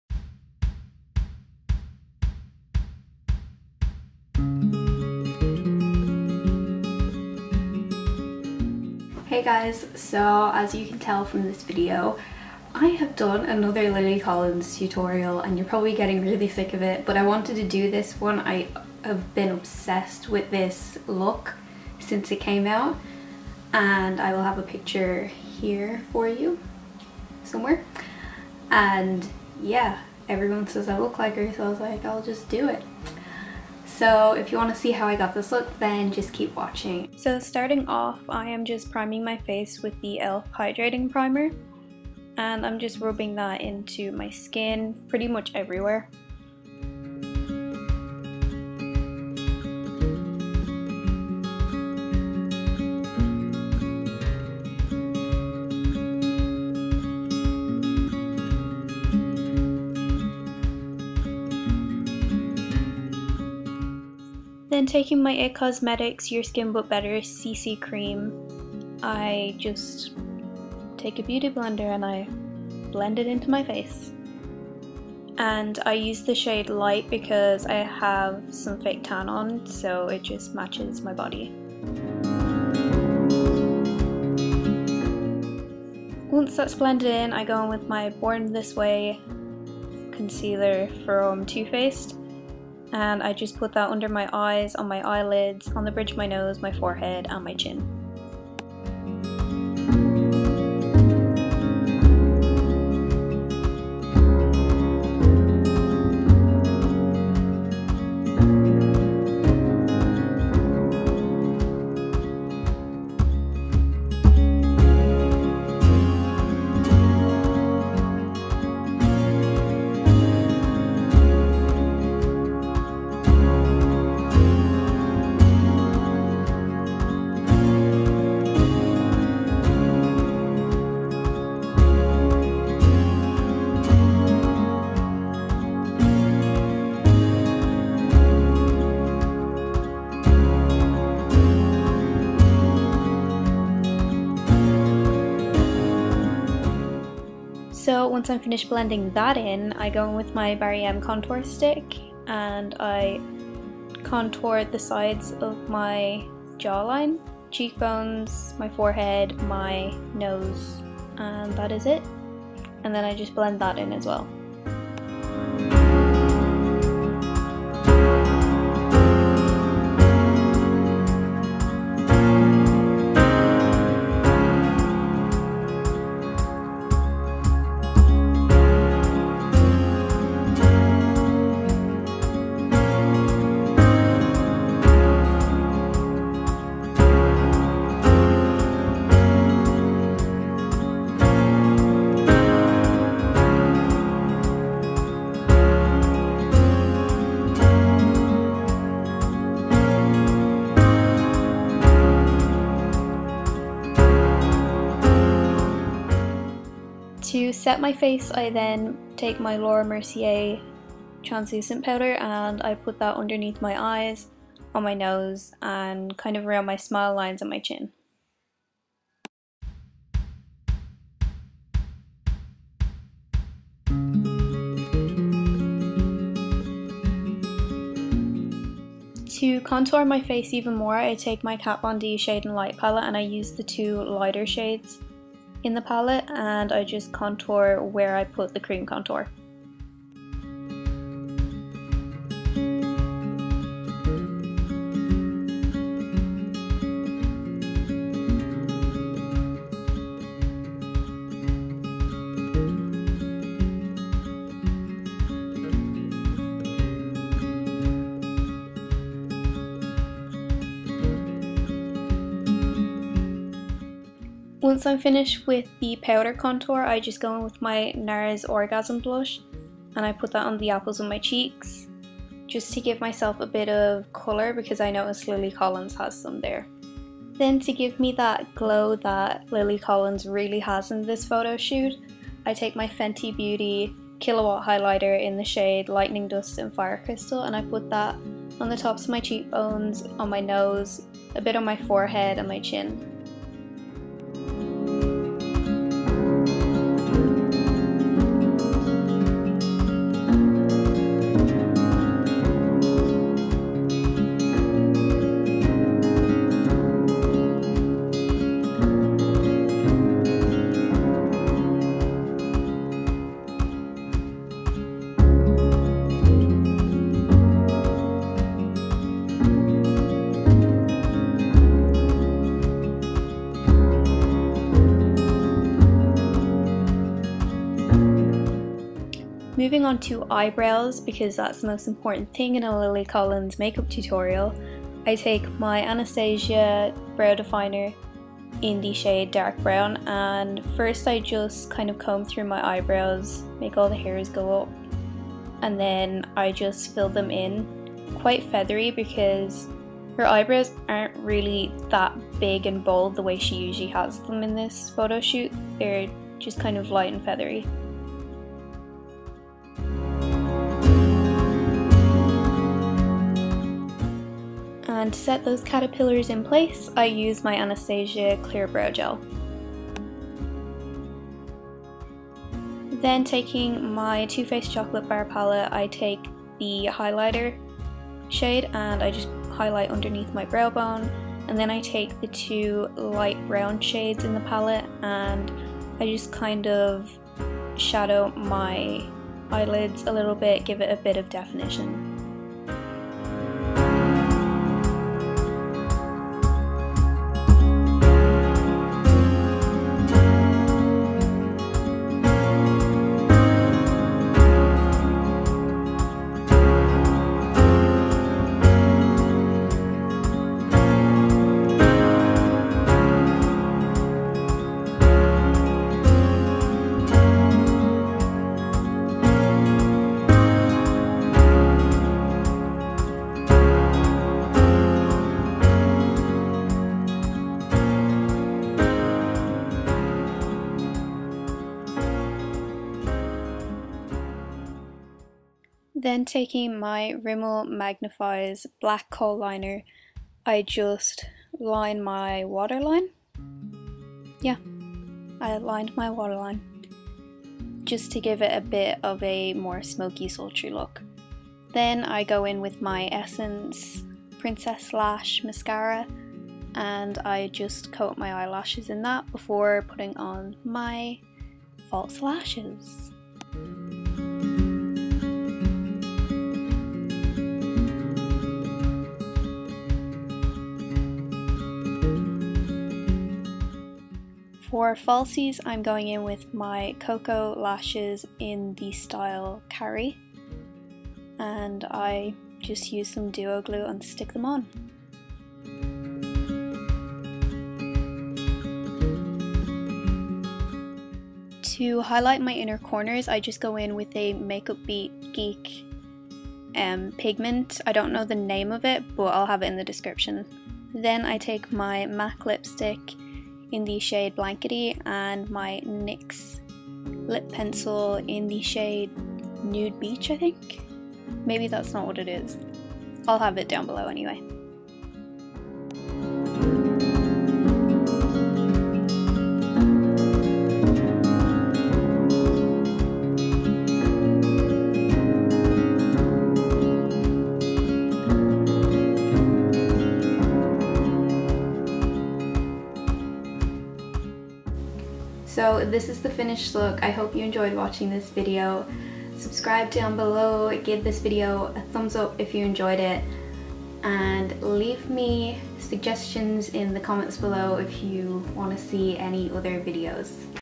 LIly Collins Malibu Mag Makeup Tutorial
Hope you enjoyed this video :) have a lovely day :)

PRODUCTS USED :
Elf cosmetics Hydrating primer
It Cosmetics Your skin but better illuminating CC cream in light
TooFaced Born this way concealer
Barry M Contour stick
Fenty Beauty Killwatt Highlight in fire crystal and lightning dust
ABH brow definer in dark brown
ABH clear brow gel
TooFaced Chocolate Bar palette
Rimmel Magnif'eyed black liner
Essence Lash princess Mascara
Koko lashes in style Carrie
Nyx Nude lip liner
MAC creme lipstick in Blankety